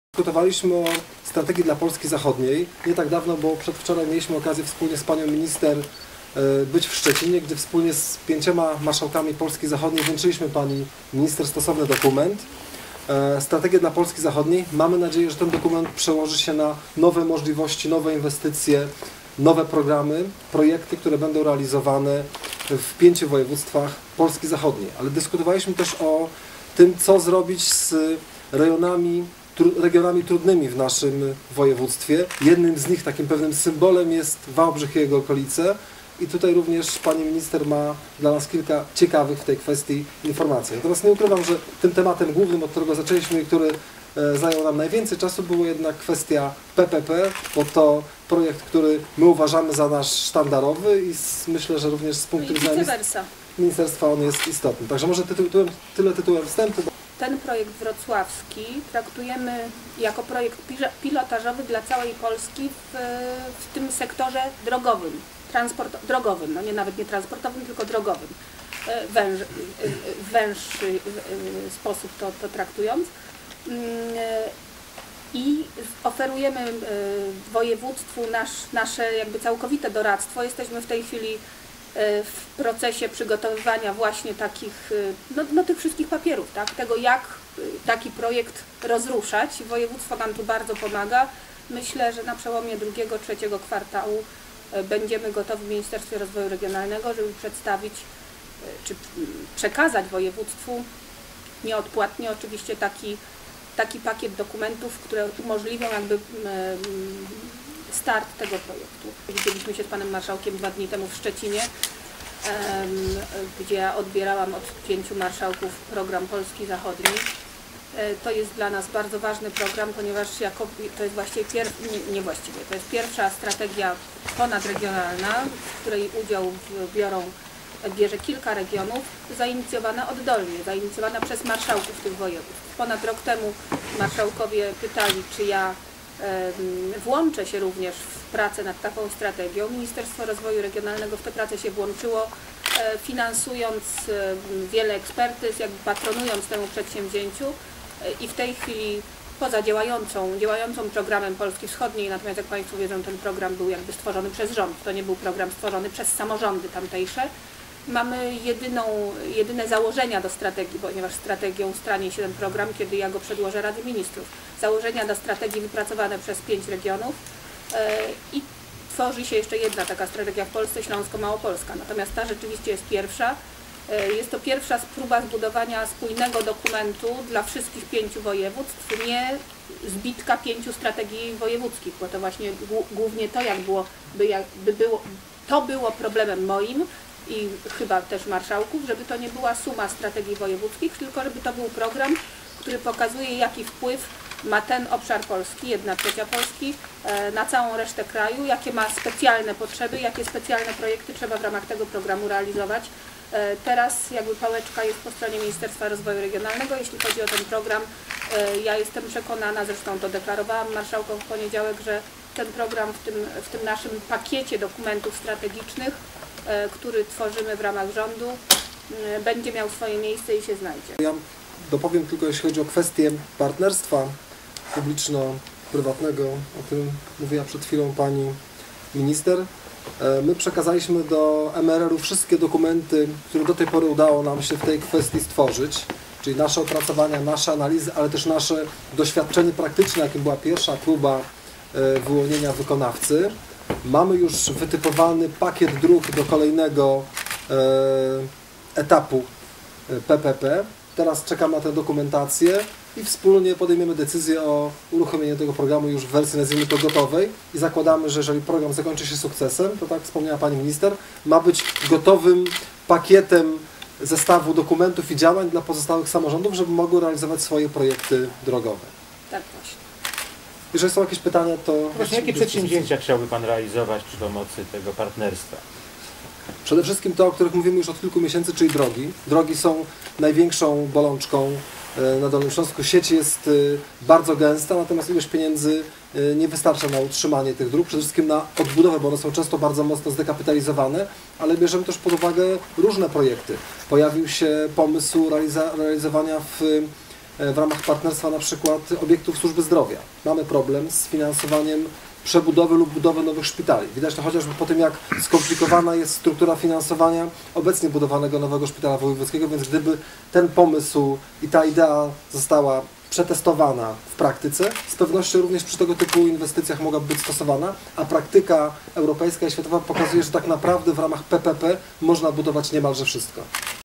Wizyta minister Elżbiety Bieńkowskiej
4.kwietnia 2012 roku z wizytą na Dolnym Śląsku przebywała minister rozwoju regionalnego Elżbieta Bieńkowska. Konferencja prasowa z marszałkiem Rafałem Jurkowlańcem odbyła się w Dolnośląskim Centrum Filmowym. Więcej informacji o wydarzeniu